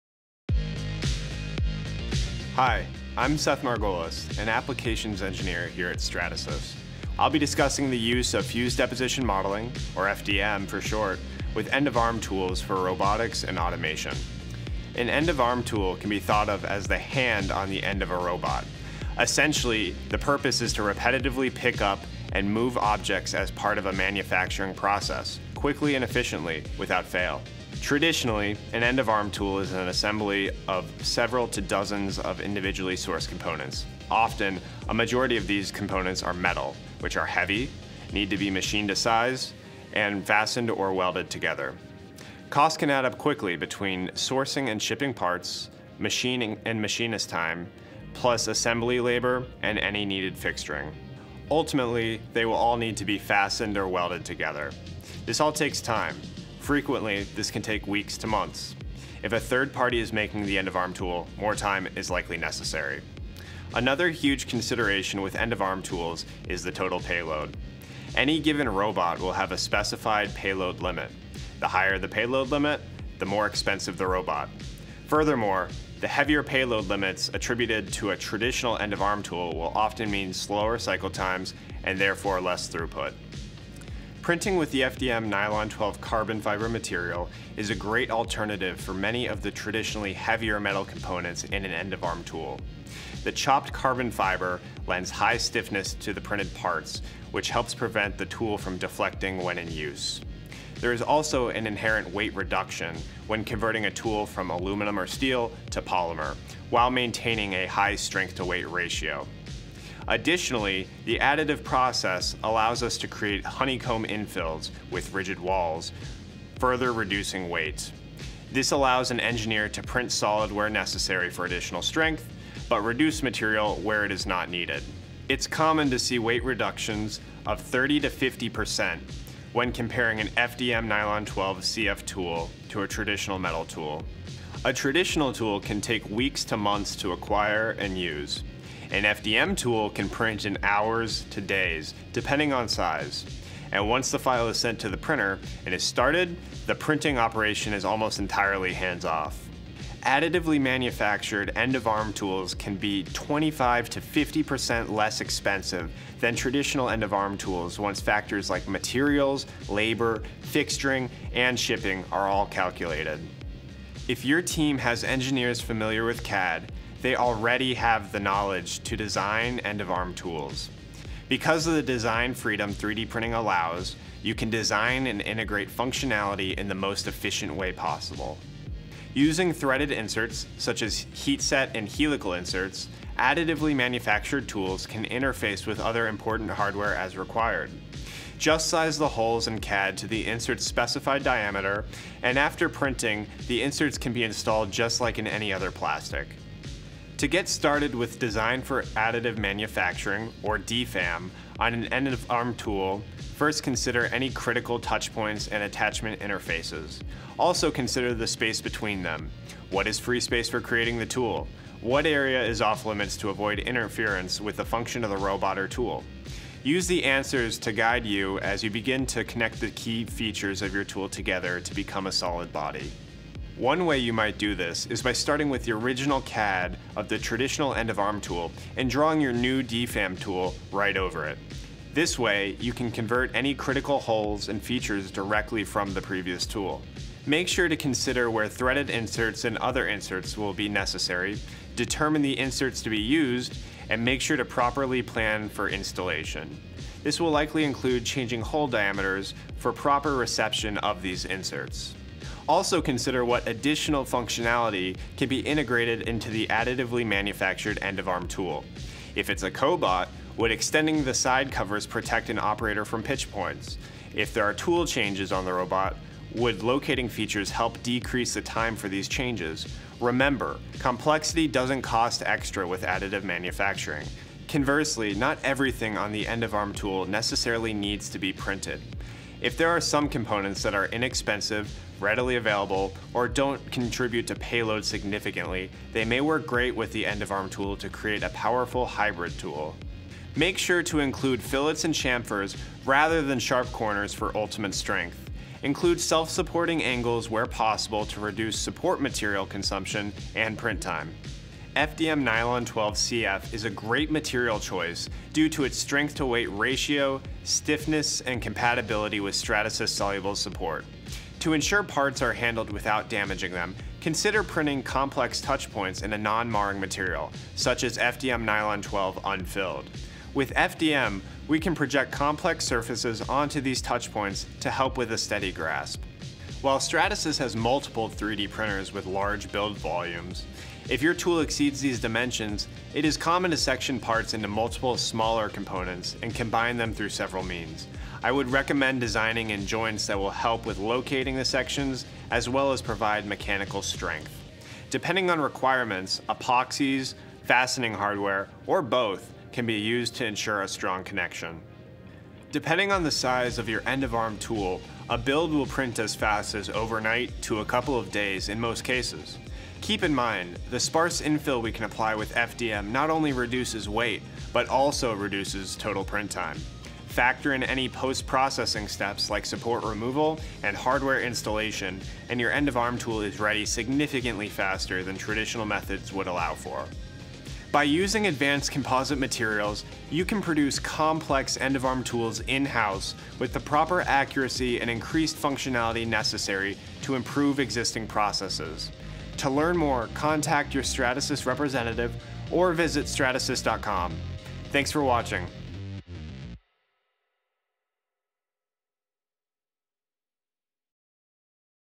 Cut Robotic EOAT Weight by up to 50% With FDM Technology
3D printed robotic end effectors are lighter, faster to make and less costly than their metal counterparts. They can also operate with smaller robots. Watch the video to learn the benefits of FDM EOATs and how to make them.